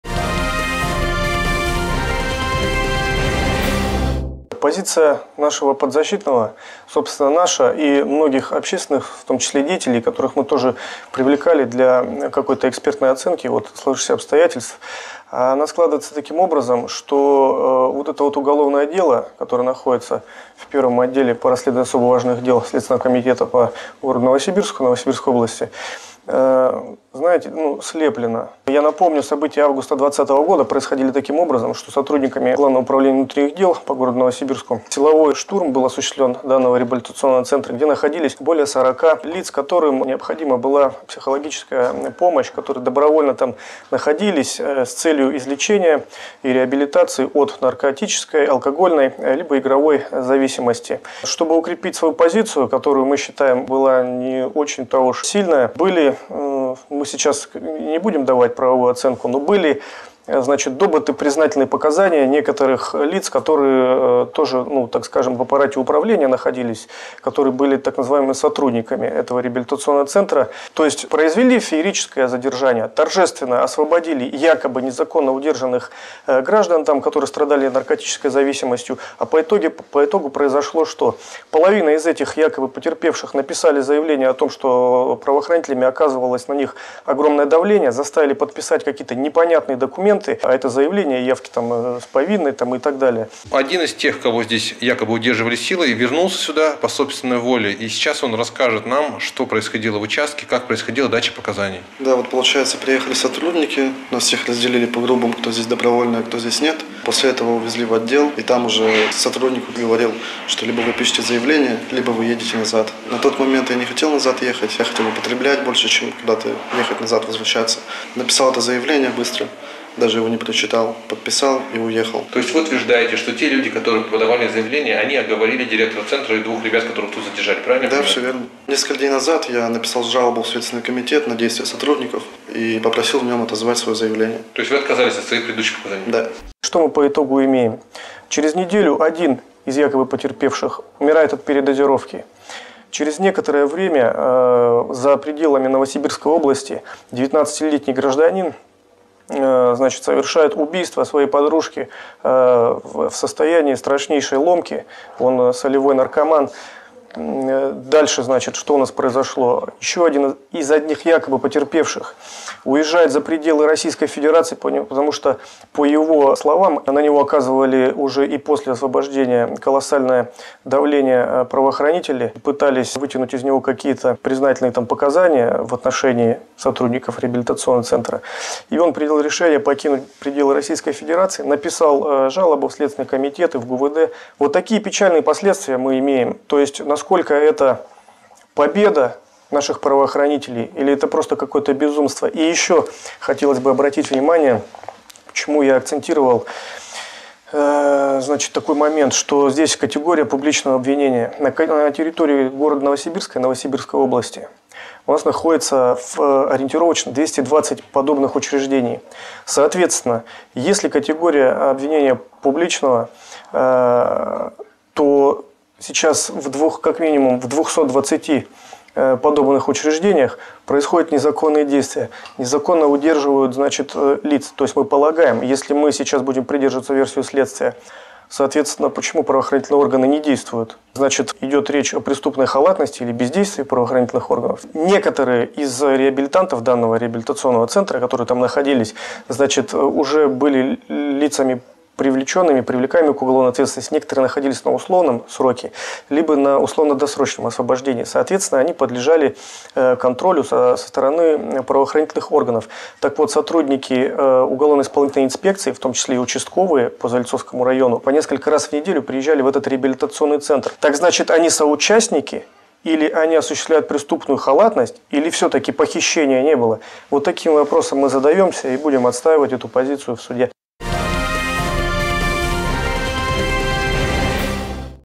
Юрист об уголовном деле о "незаконном удержании" 25 пациентов реабилитационного центра
Год назад следователи с участием СОБРа триумфально освободили пятьдесят пациентов ребцентра «Национальный проект». Спустя год следствия были получены показания 25 человек, заявивших, что их удерживали насильно. Однако это дело не так однозначно, как может показаться на первый взгляд.

По ссылке: полное заявление СК по данному делу. Развернутое интервью представителя директора "Национального проекта", которого го держат в СИЗО без предъявления обвинений. Эксклюзивные документы и интересные подробности, видеосюжет для телеэфира.